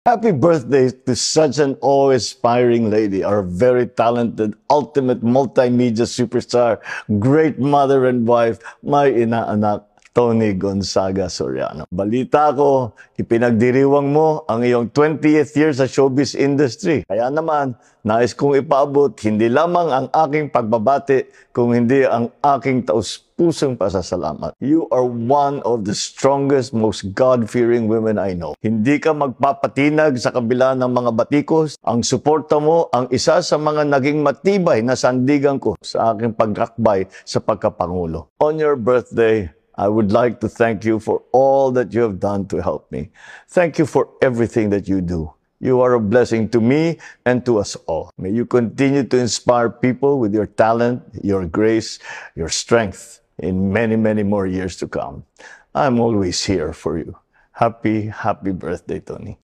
PBBM kinilala si TONI GONZAGA sa ambag nya noong eleksiyon#pbbm
Malaking bahagi si TONI G.sa  campain ni PBBM noon sa eleksiyo kaya sinurpresa nya ito sa kanyang bday concert.@ToniGonzagaStudio